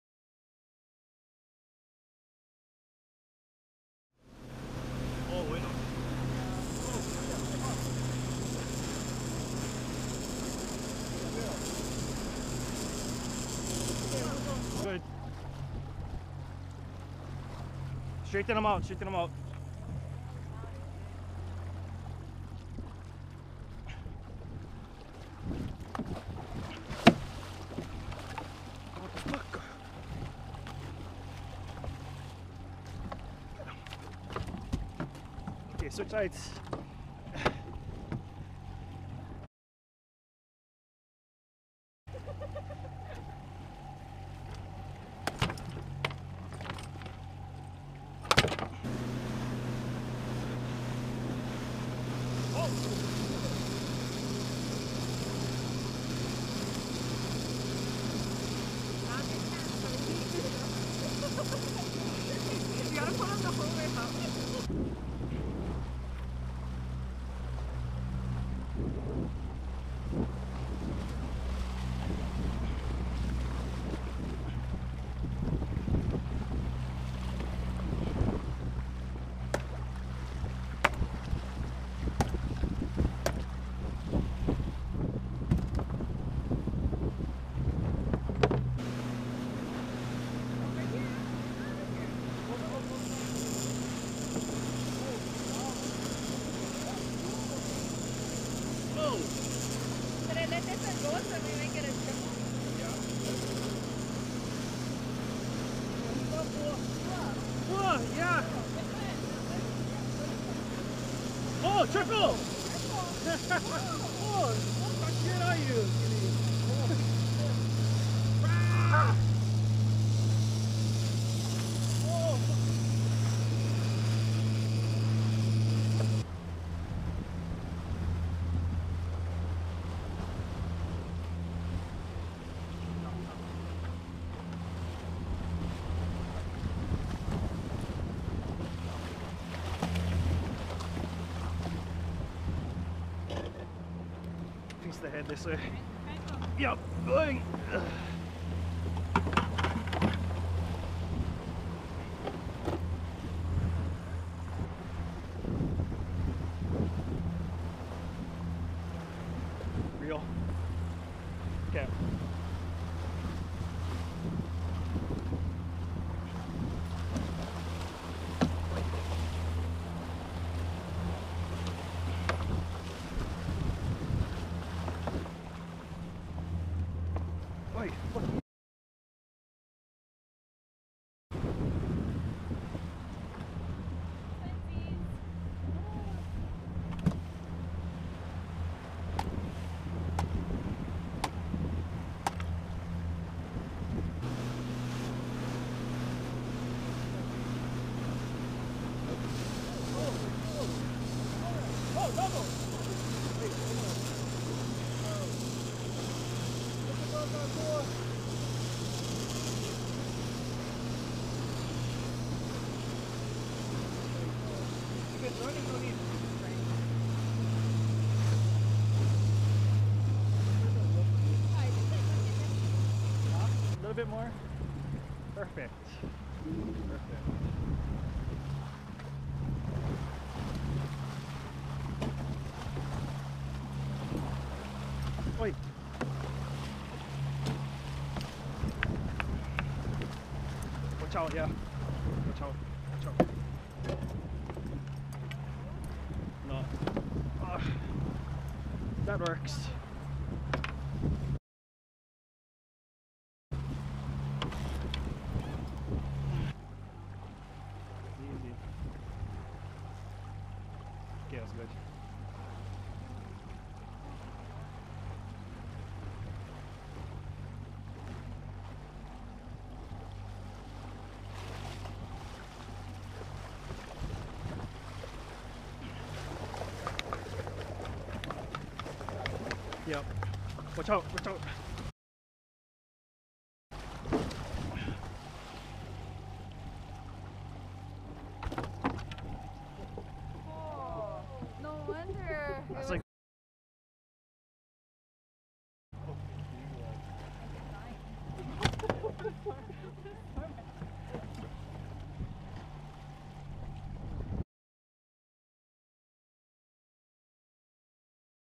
2015 Ep #5- Six Ono Wahoo Fishing Hawaii GoPro
Had some visitors from Kaua'i and was lucky enough to put them on some fish.